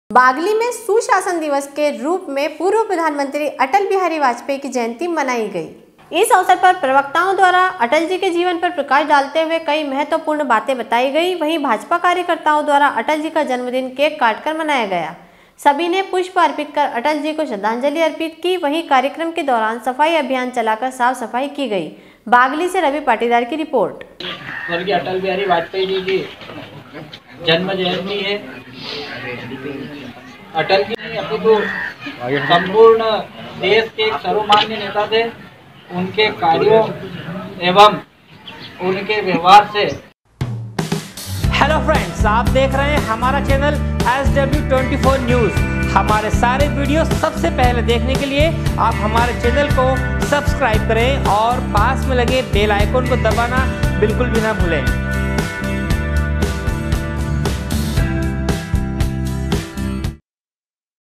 Bagli- बागली में सुशासन दिवस के रूप में पूर्व प्रधानमंत्री अटल विहारी वाजपेई की जयंती मनाई।
बागली में सुशासन दिवस के रूप में पूर्व प्रधानमंत्री अटल विहारी वाजपेई की जयंती मनाई। बागली से रवि पाटीदार की रिपोर्ट।